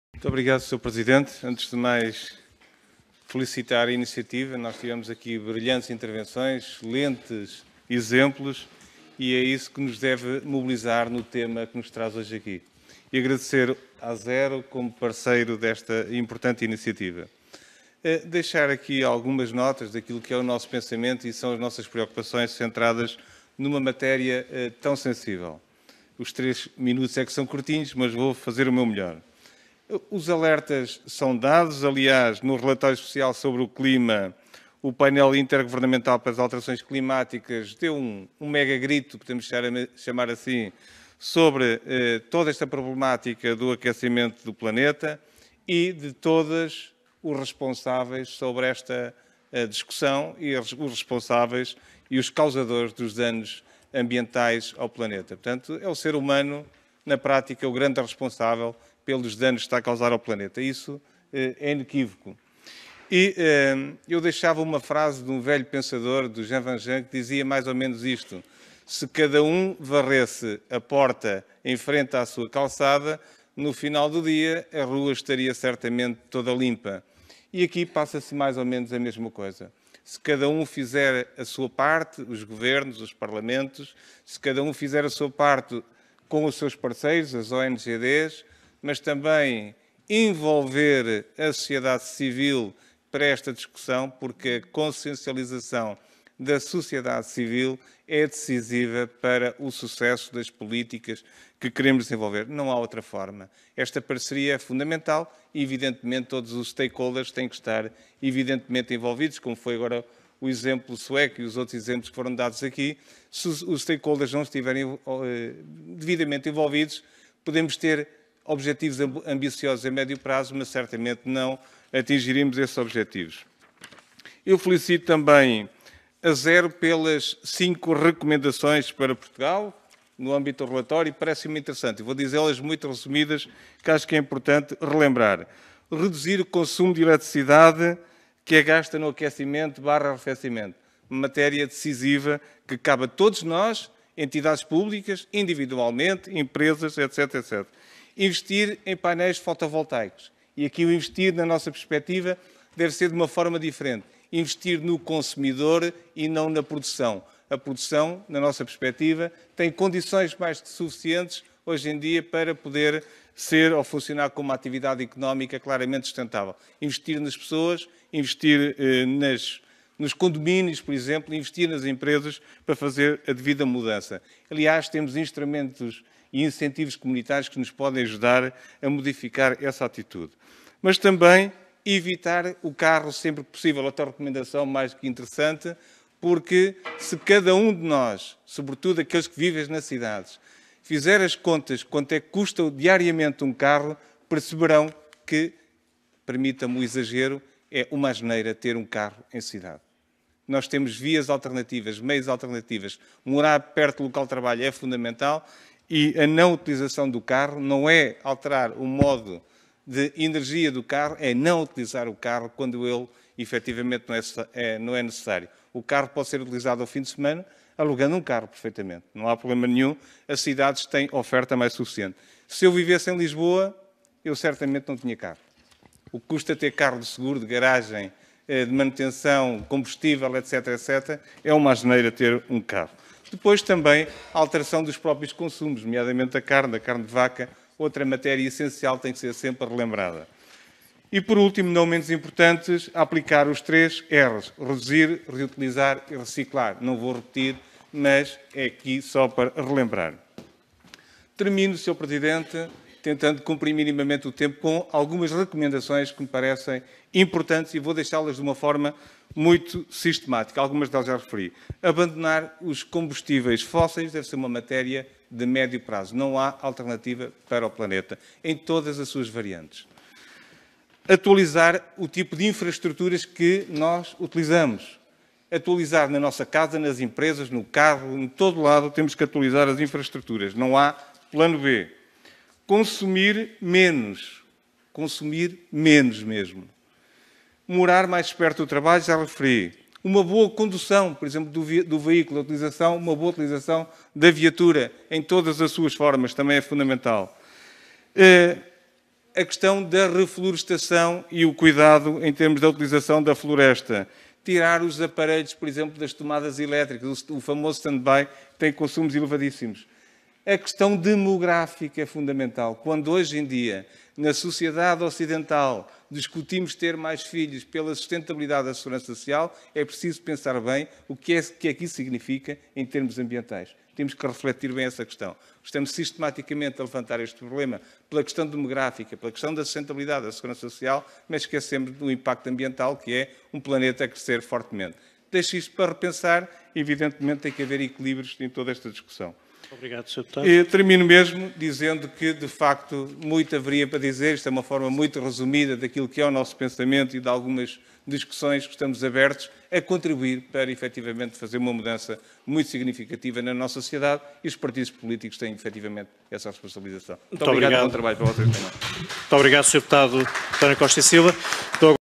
António Costa Silva na Conferência: "Oportunidade para uma lei de Bases do Clima"
Intervenção na conferência organizada pela Comissão de Ambiente, Ordenamento do Território, Descentralização, Poder Local e Habitação.